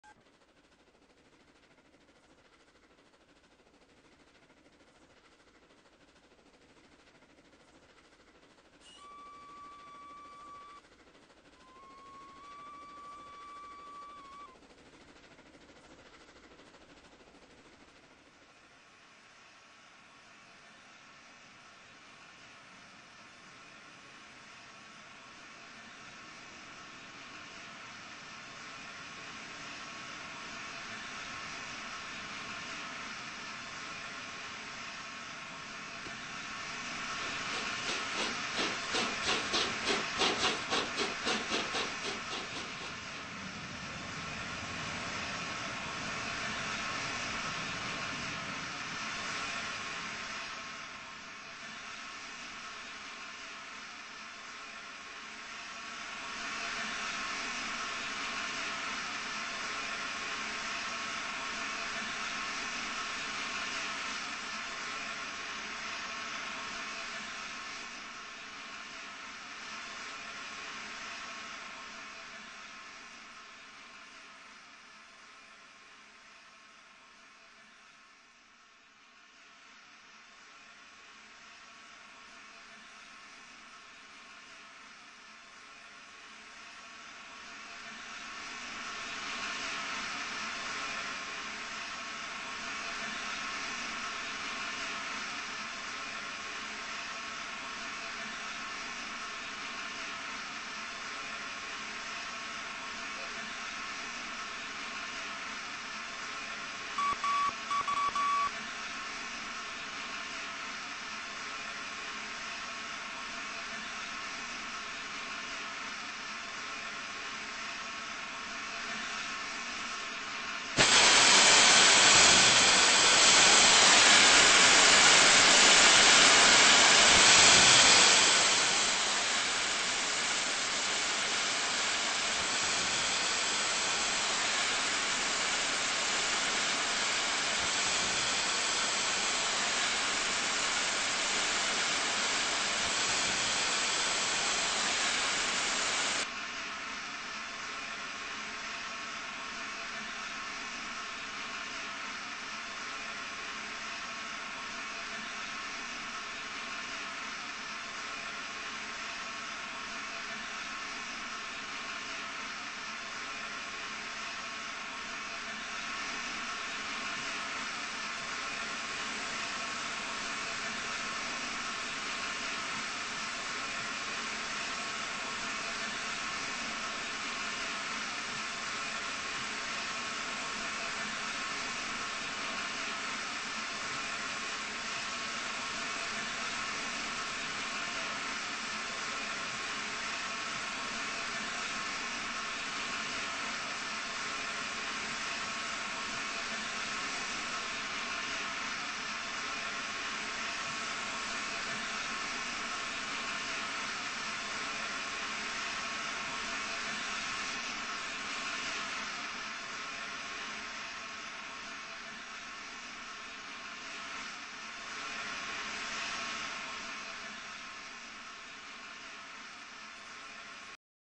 GCR tornado
just a look at the gcr tornado made by themodsterRB